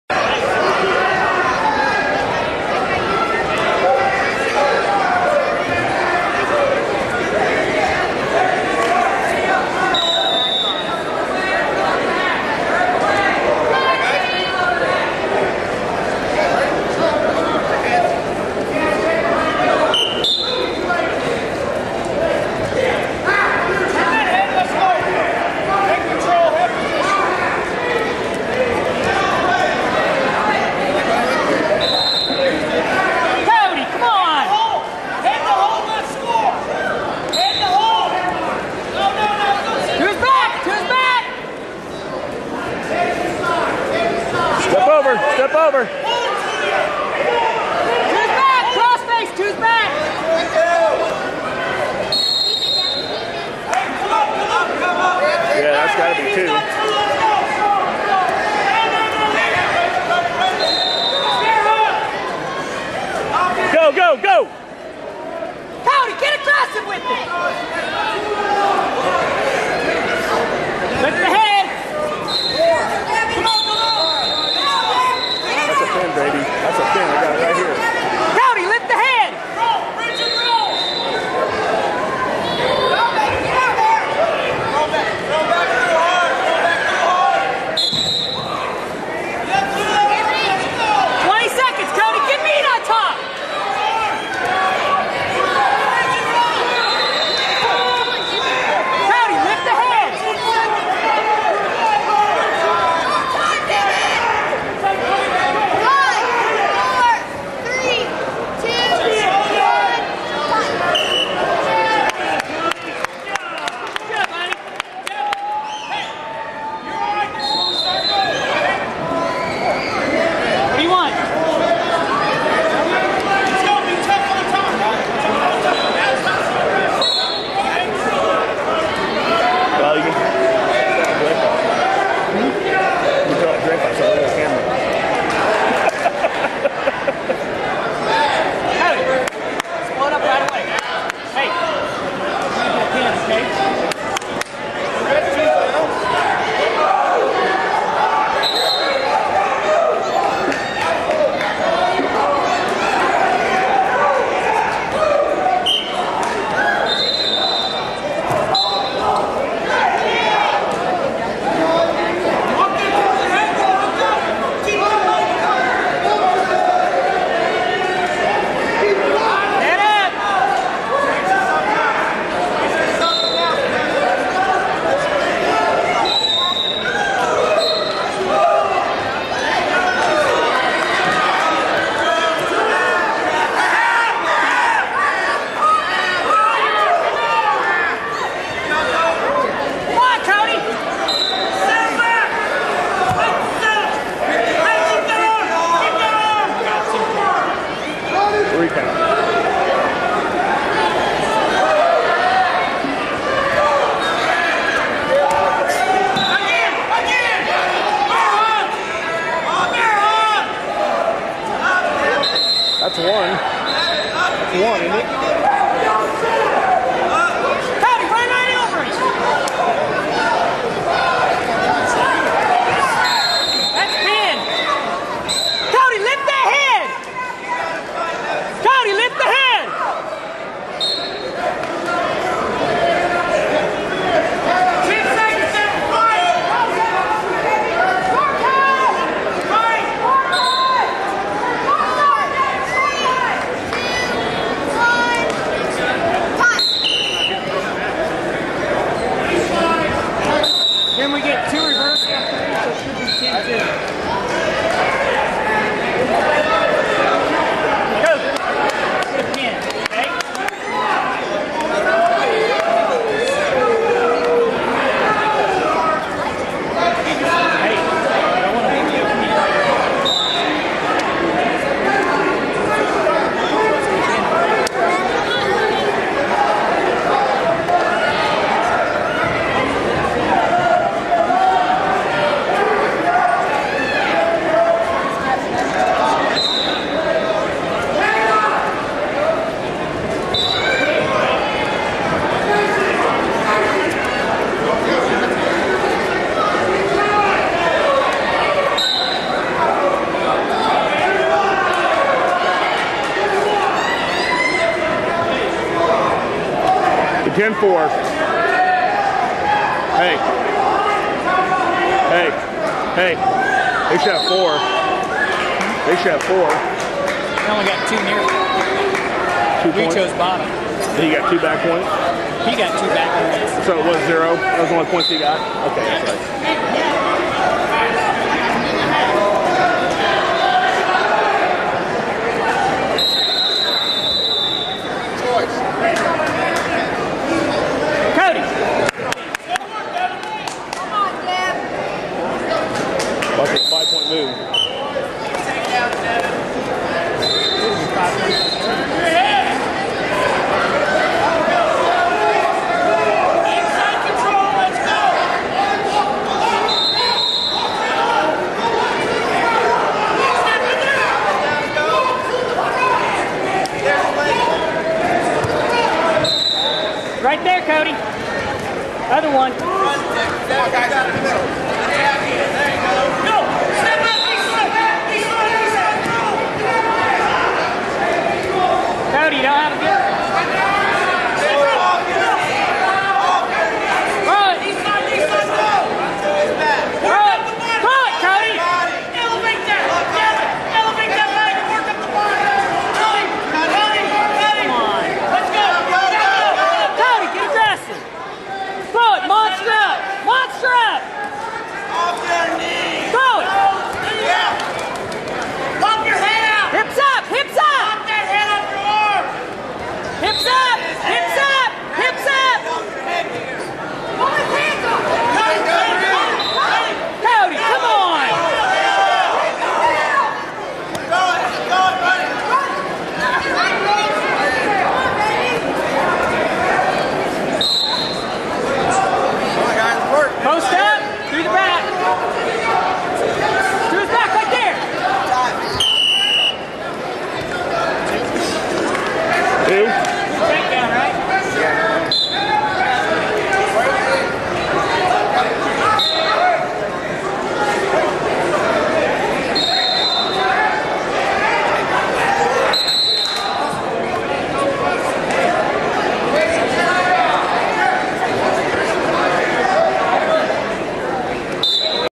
Cody Phippen (KS) vs Devon Gulledge (MO) Park Hill 2009
Cody Phippen of the East Kansas Wrestling Club wrestling Missouri State
Champion Devon Gulledge at the Park Hill Tournament 2009.